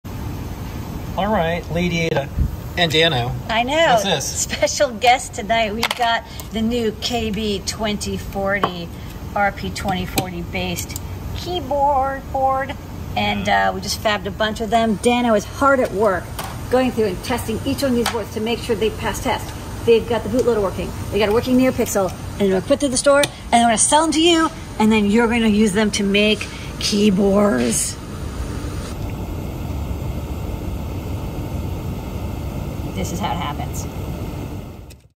👀🐷👀 KB2040 coming sooooonn..... 🐷👀🐷
This week's new product feature is the KB2040, we're hard at work testing these boards that we fabbed Monday morning. This tester takes only about 3 seconds so we can crank through all these keebers and get them into the shop ASAP. sign up now to get notified the moment we put them in stock